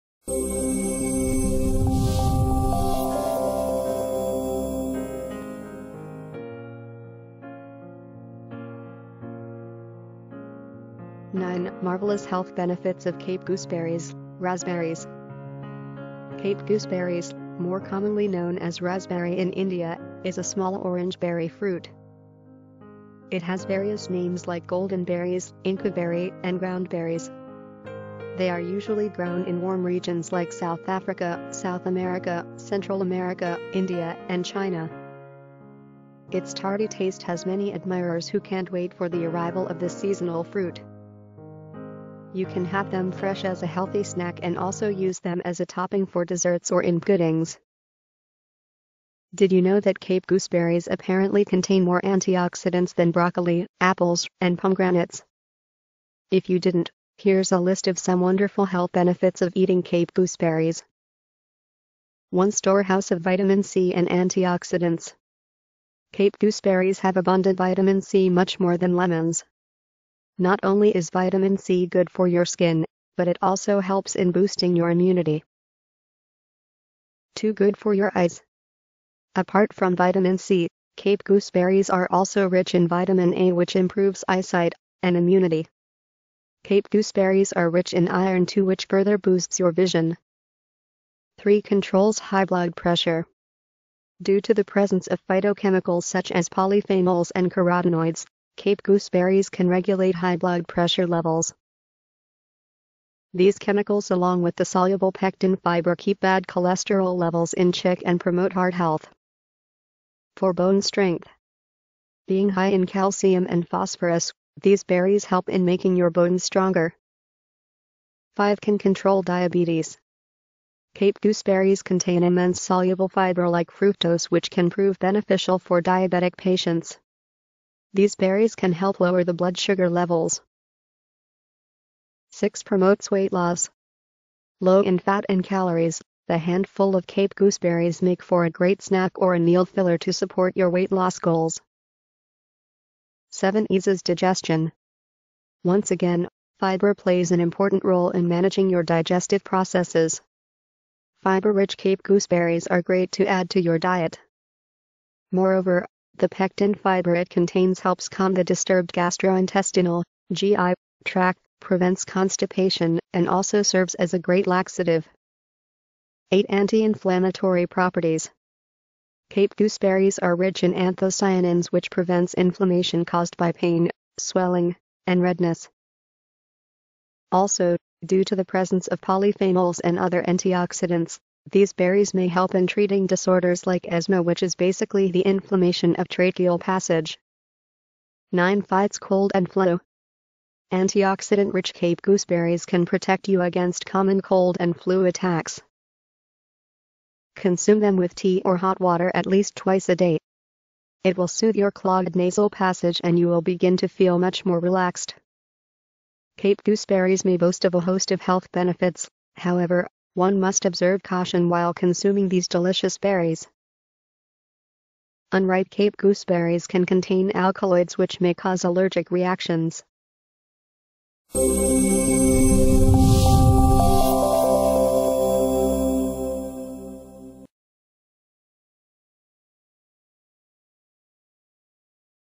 9 Marvelous Health Benefits of Cape Gooseberries
9 Marvelous Health Benefits of Cape Gooseberries
Cape gooseberries, more commonly known as Rasbhari in India, is a small orange berry fruit. It has various names like golden berries, inca berry and ground berries. They are usually grown in warm regions like South Africa, South America, Central America, India and China. Its tarty taste has many admirers who can’t wait for the arrival of this seasonal fruit. You can have them fresh as a healthy snack and also use them as a topping for desserts or in puddings. Did you know that cape gooseberries apparently contain more antioxidants than broccoli, apples, and pomegranates? If you didn't, here's a list of some wonderful health benefits of eating cape gooseberries.
1. Store house of Vitamin C and antioxidants
2. Good for your eyes
3. Controls high blood pressure
4. Bone strength
5. Can control diabetes
6. Promotes weight Loss
7. Eases digestion
8. Anti-Inflammatory properties
9. Fights cold and flu